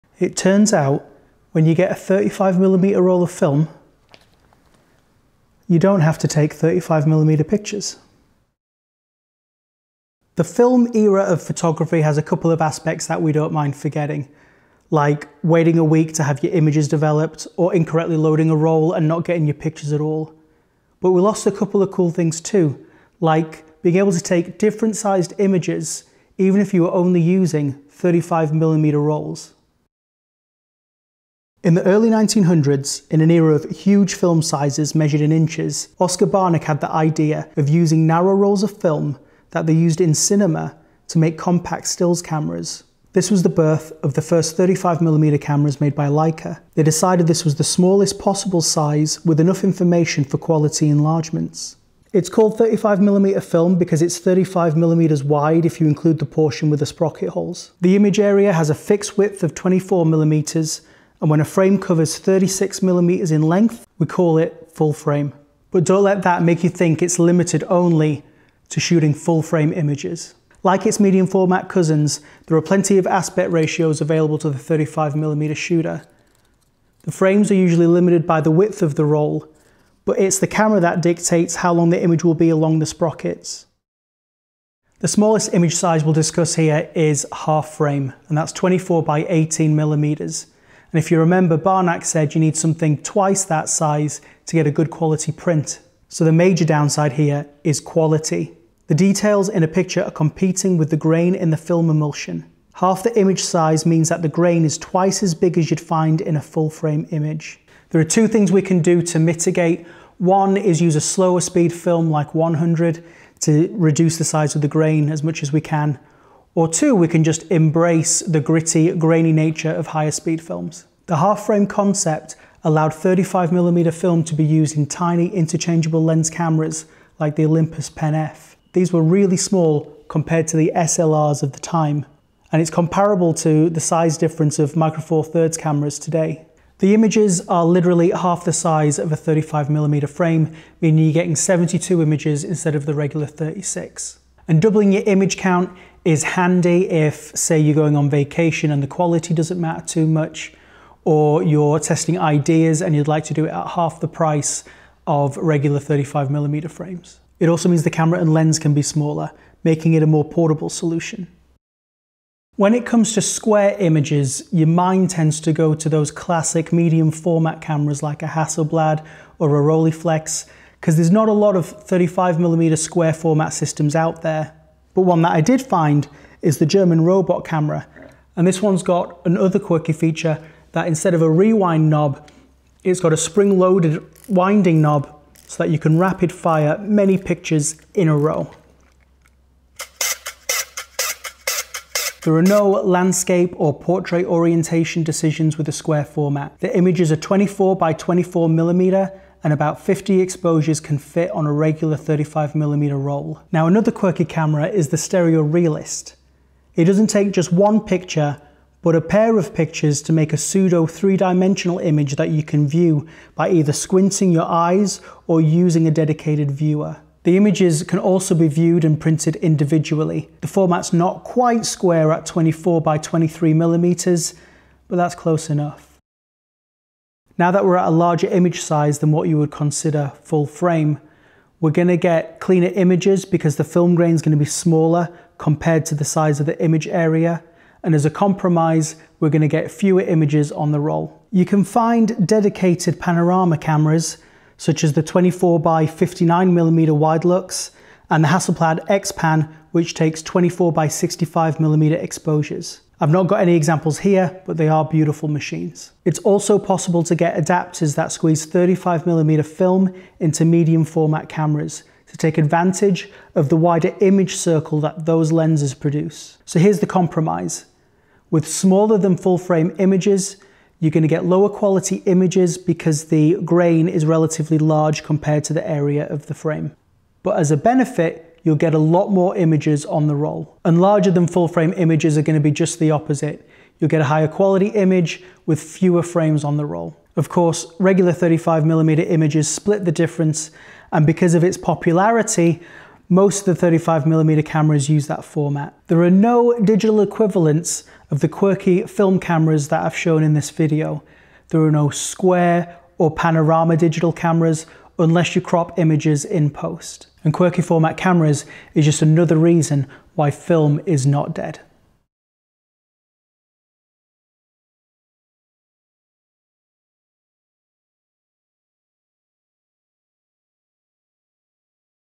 There's MORE to 35mm film than just 35mm photos...
Sensor size in digital cameras is fixed, but with film you can get different sized frames by using different cameras, even if you only use rolls of 35mm film. We'll explore some of these quirky cameras and the different sized images they produce.